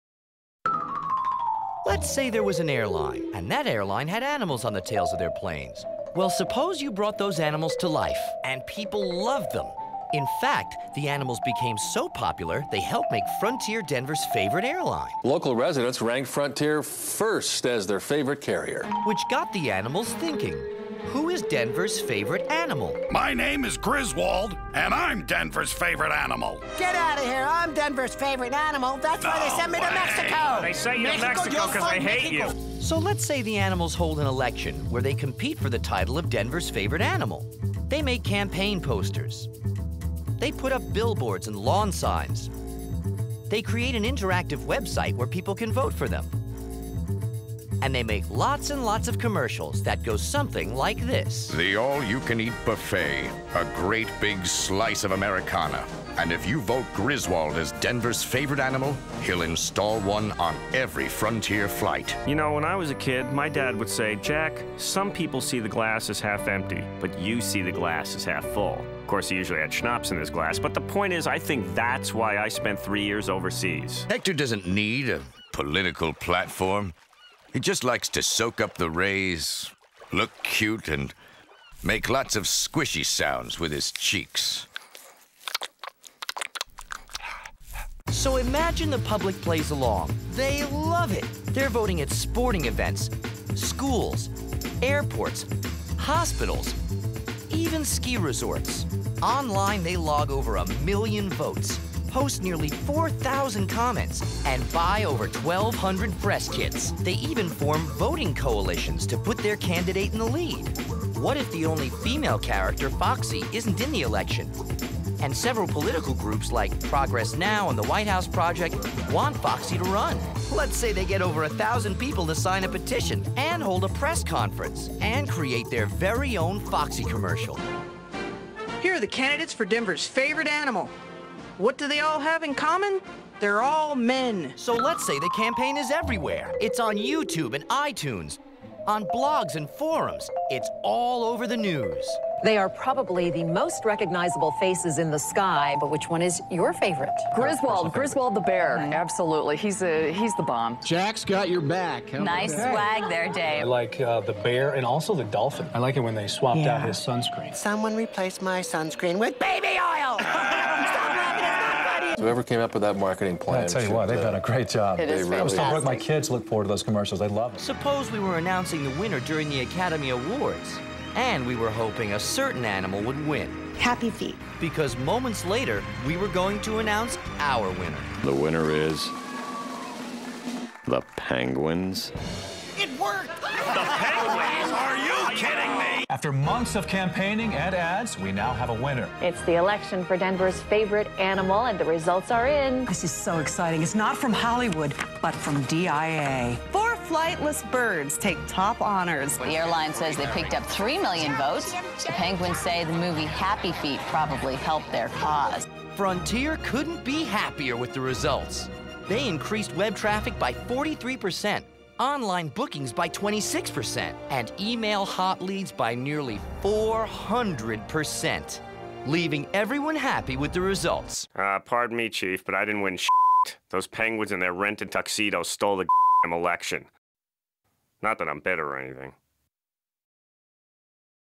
Frontier Airlines: Election Film
When Frontier's animals talk, the community answers. This short film demonstrates how we used traditional and non-traditional media to create a two-way conversation with the Denver market - blurring the line between advertising, news and popular culture.